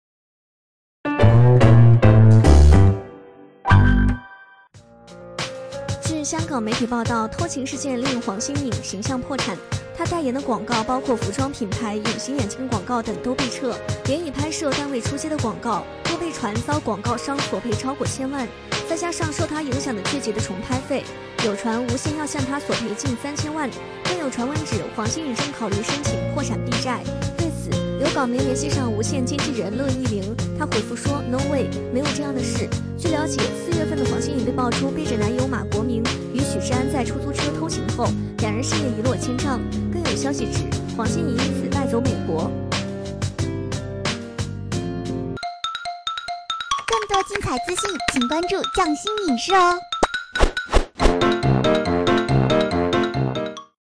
黄心颖将申请破产 经纪人否认称没有这样的事
黄心颖和许志安事情被报道之后，黄心颖的很多广告被撤，还要申请破产抵押，经纪人出言否认这样的事情